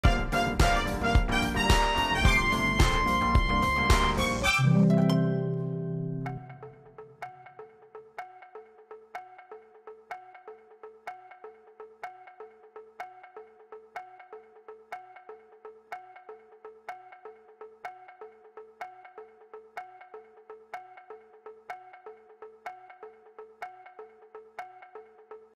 Can You Solve This Connection? Guess the cricketer
Guess the cricketer! Put your thinking cap on! 🧢 Can you guess the word that links these 4 pictures? Test your puzzle-solving skills with our 'Connection' challenge. Ready for some brain exercise? 🤔